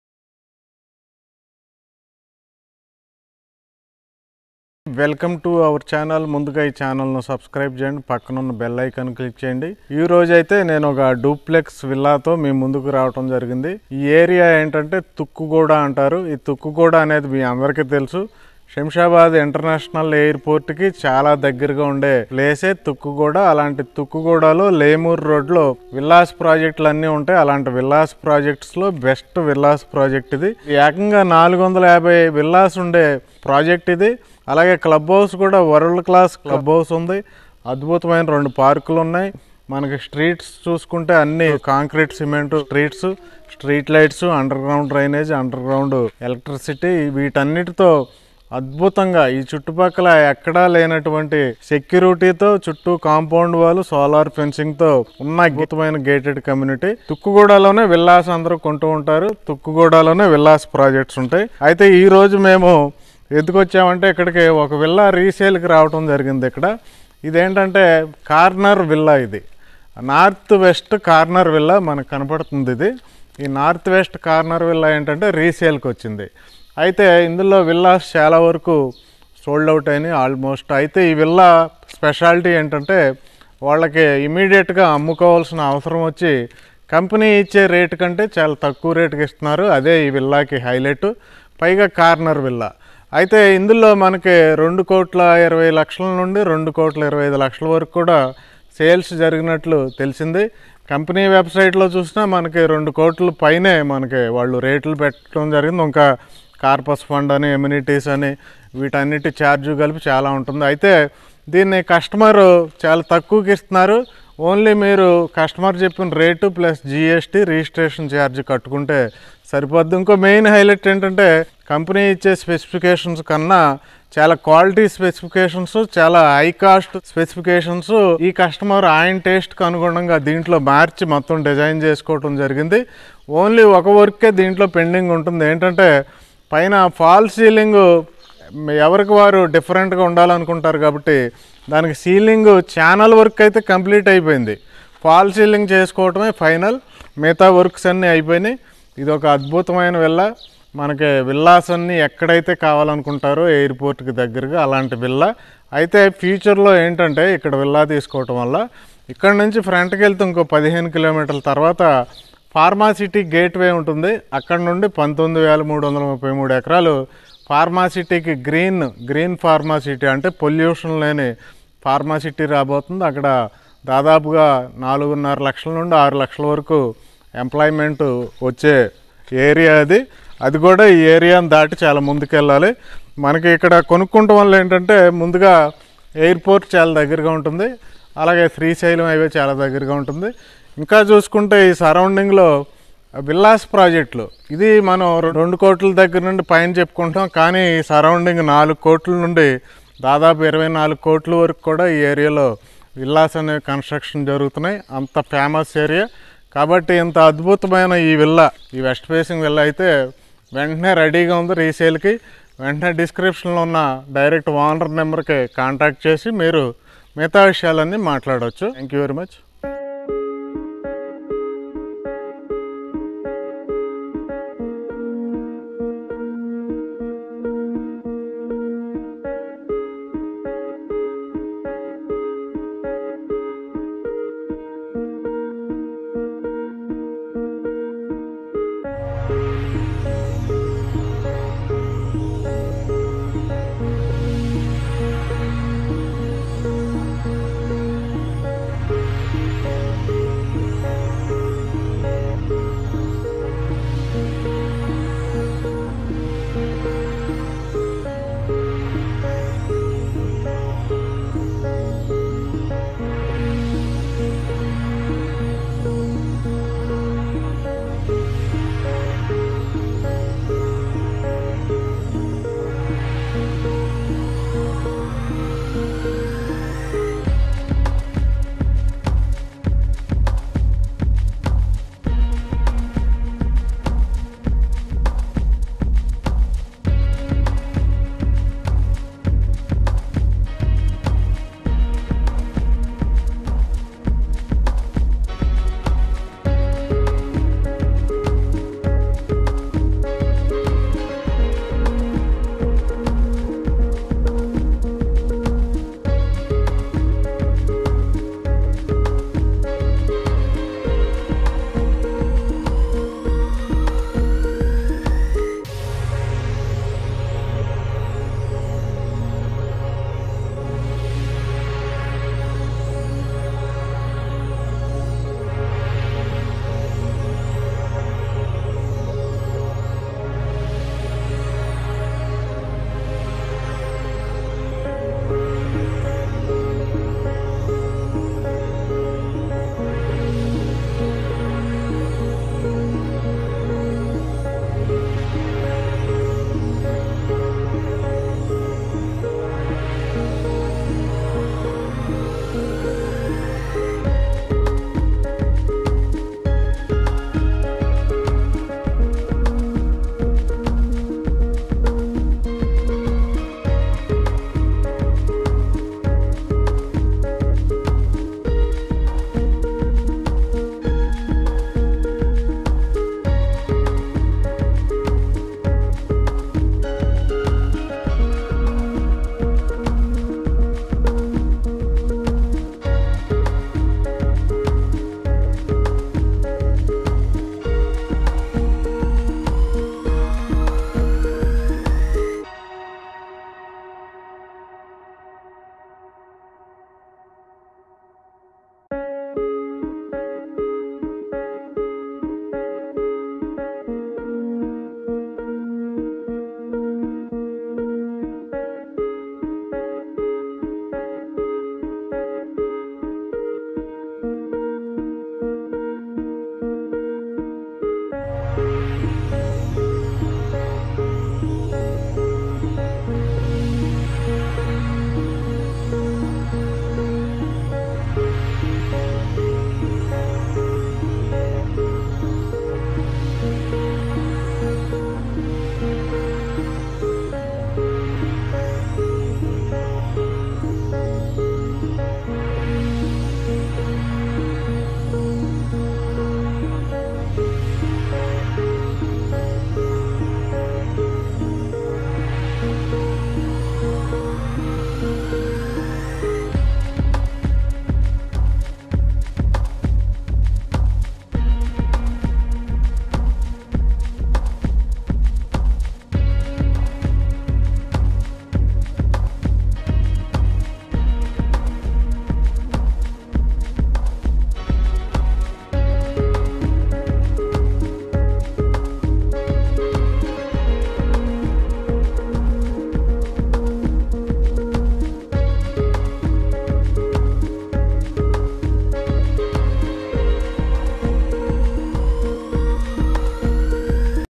READY TO OCCUPY VILLAS TUKKUGUDA VILLAS VILLAS NEAR AIRPORT || DUPLEX VILLAS IN TUKKUGUDA
READY TO  OCCUPY VILLAS  TUKKUGUDA VILLAS  VILLAS NEAR AIRPORT  || DUPLEX VILLAS IN TUKKUGU

Luxury Duplex Villa For sale in Tukkuguda Near Shamshabad International Airport at Lemur Road

Google's Location Pin

Villa Number 107

2418 Sft in 250 Sq Yards

North West Corner Villa

Kavuri Hills Forest Nest

Actual price 2.18 Crores

Offer price 2.05 Crores

Villa located in 450 Villas Luxury Gated Community

High Quality Specifications

Bath Tub in Kids Bathroom